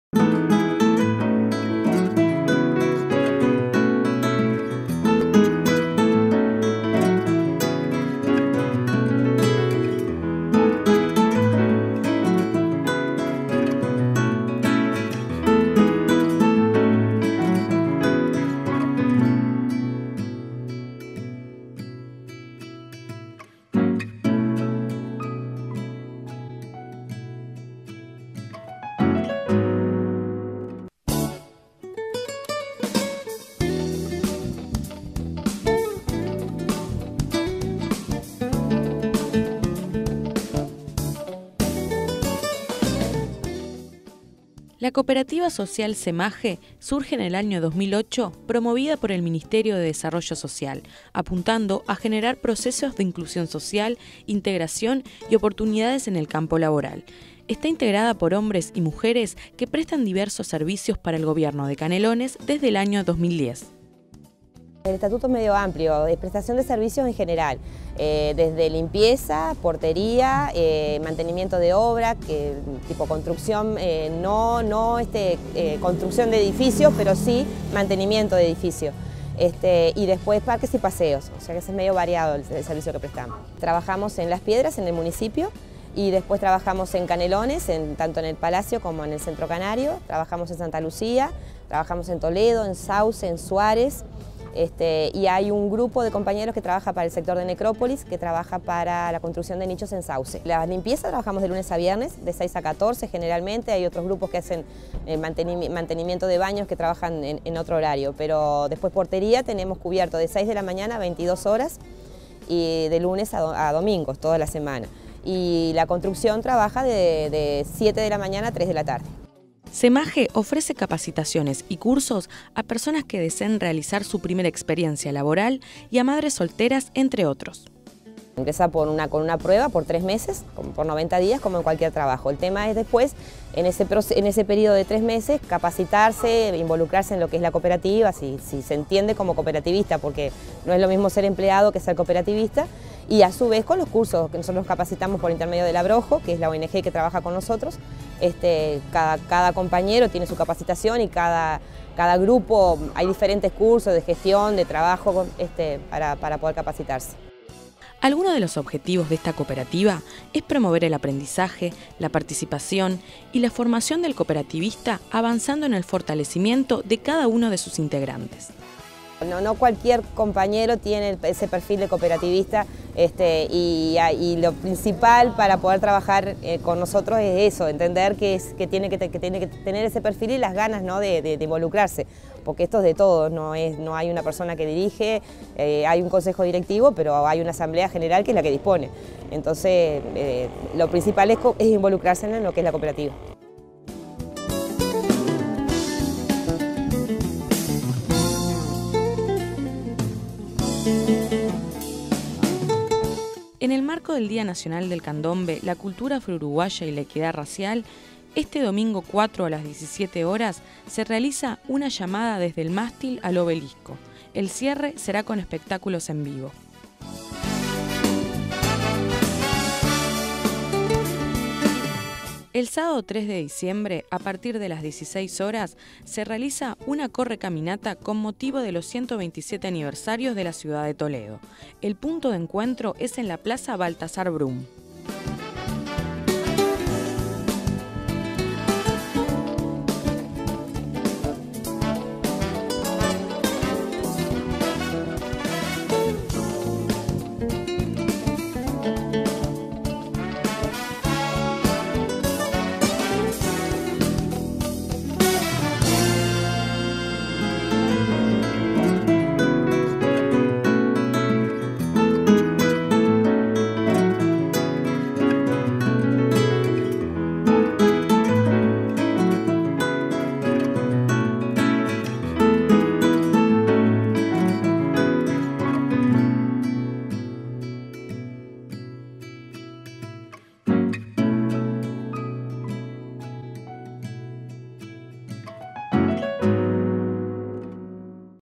Reporte Diario - Cooperativa SEMAGE 02.12.2016
En este Reporte Diario los invitamos a conocer la historia de la Cooperativa SEMAGE y el trabajo que lleva adelante en Canelones.